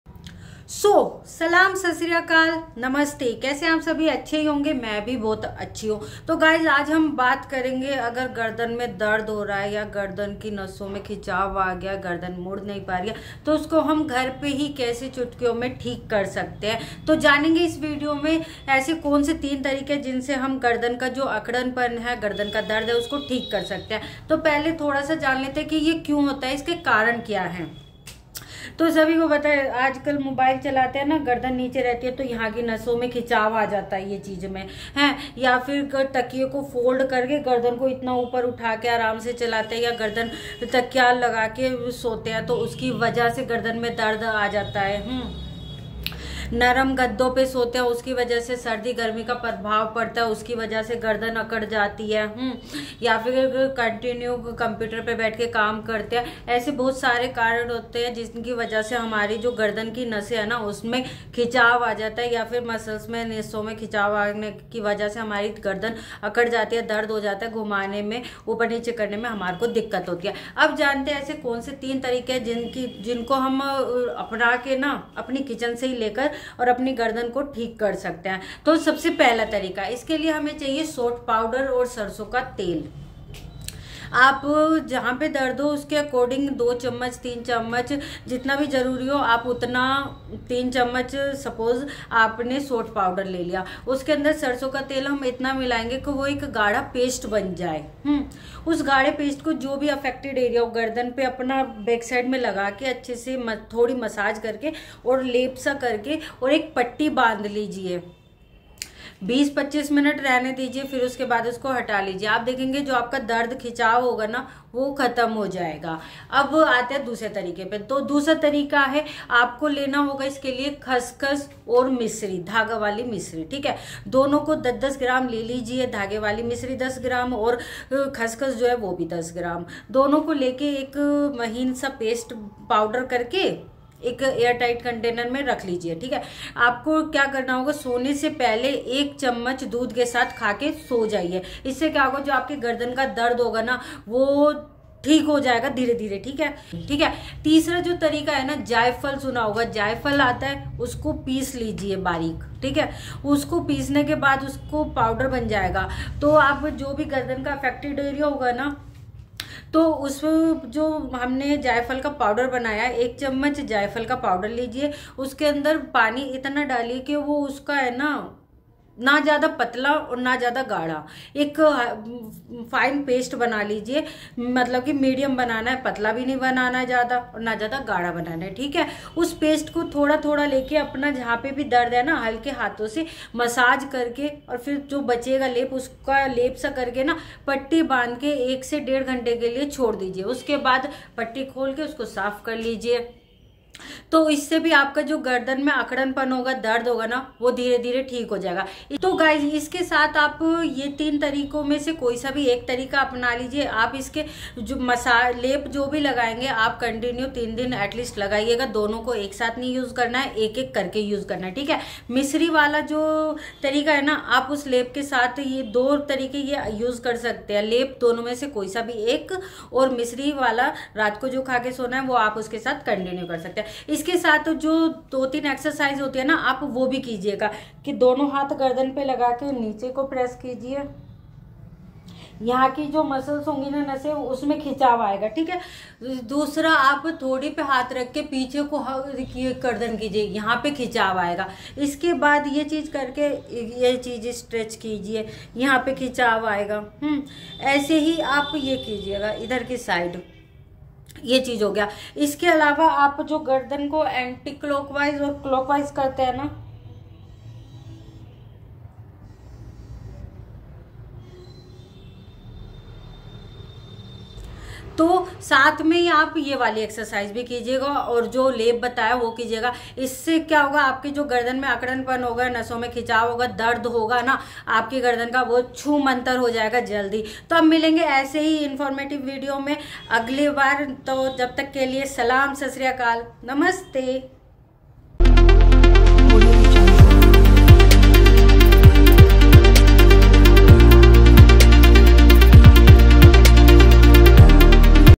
प्राकर्तिक तरीके से कैसे करे गर्दन का दर्द ठीक || Neck Pain for Home Remedies
थोड़ी ही देर में ठीक करे अकड़ी गर्दन को quick treatment for neck pain